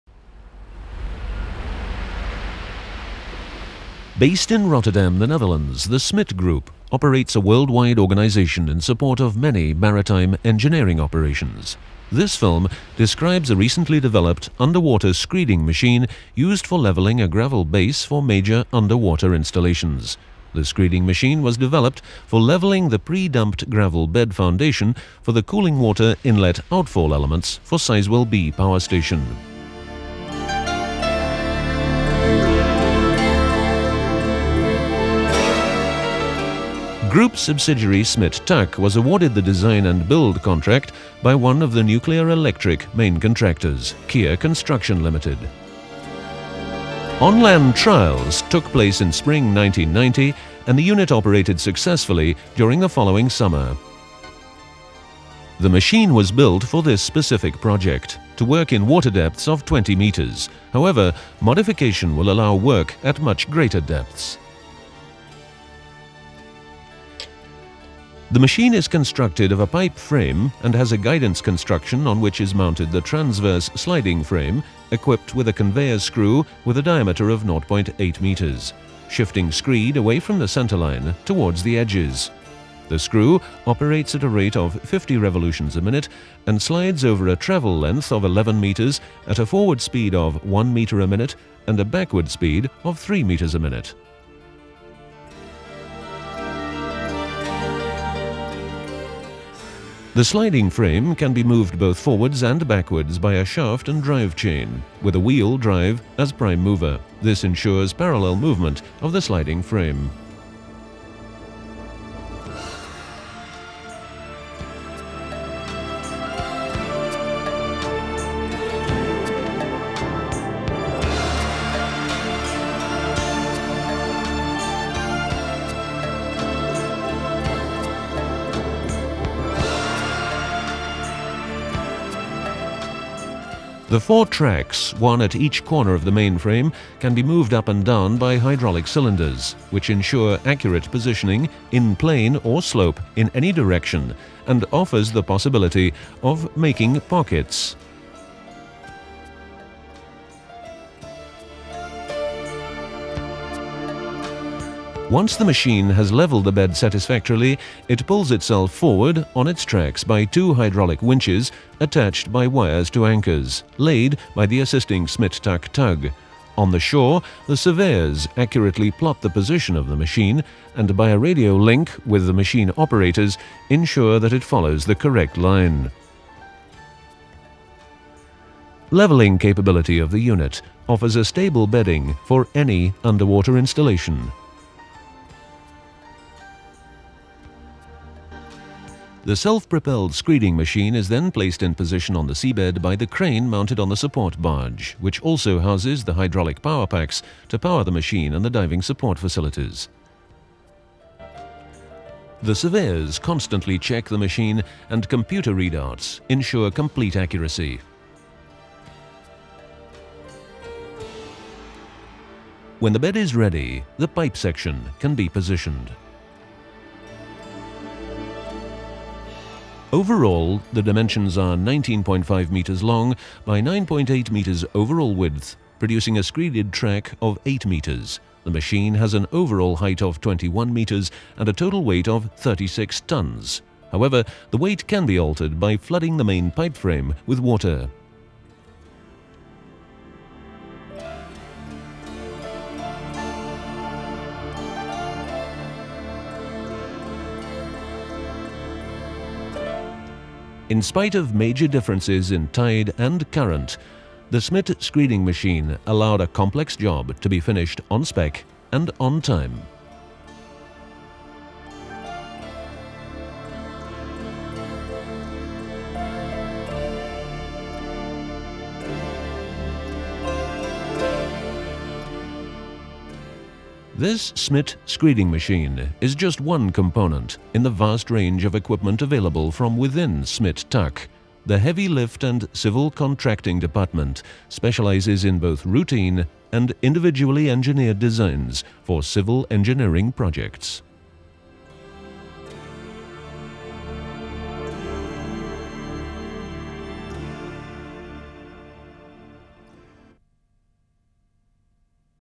Underwater Screeding Machine (Smit Tak - 1990)
In 1990 Smit Tak developed an underwater screeding machine used for levelling the gravel base for major underwater installations.

In 1990 ontwikkelde Smit Tak een machine om de gravel fundaties onder grote onderwater installaties uit te kunnen vlakken.